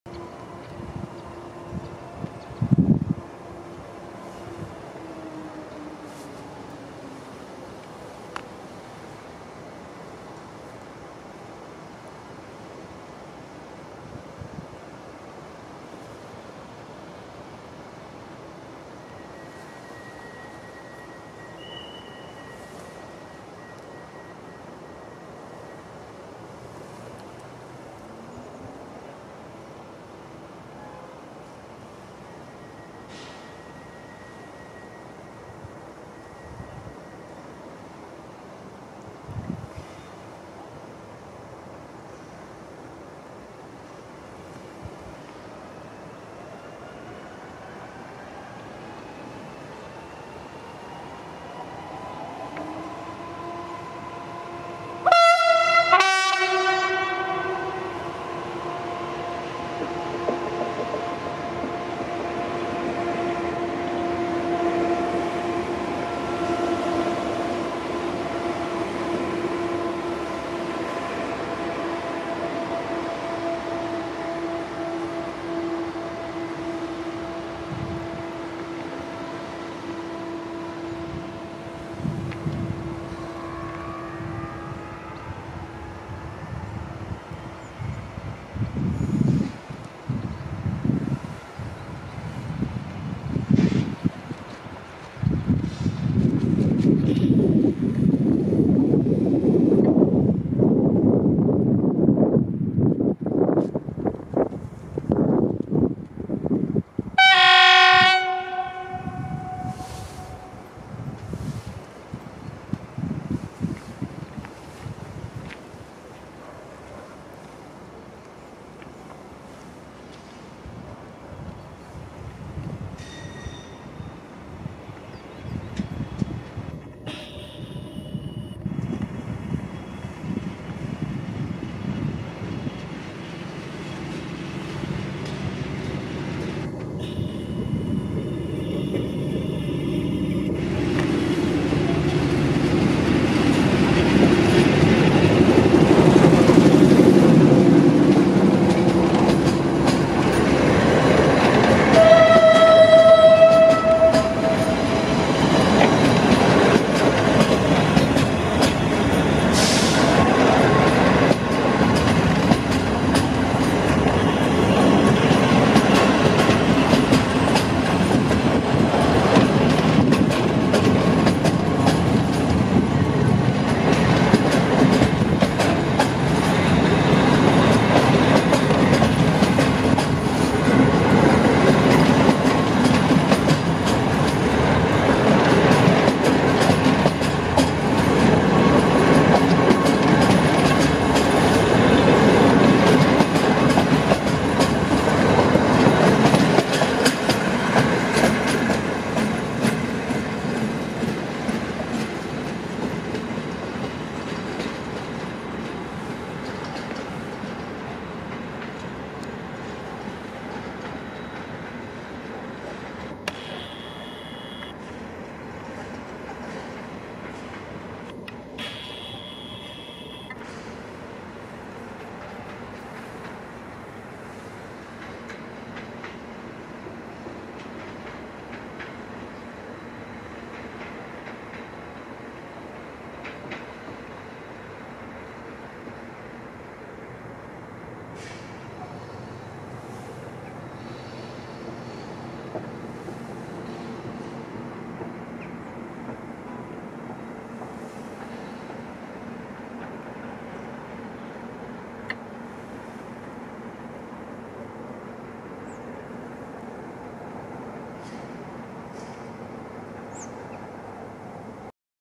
SM42-401 na klimatycznym szlaku | Linia nr 25
Godzina: 15:46

SM42-401 powolnym tempem przemierza pokryte liściami tory lini kolejowej nr 25.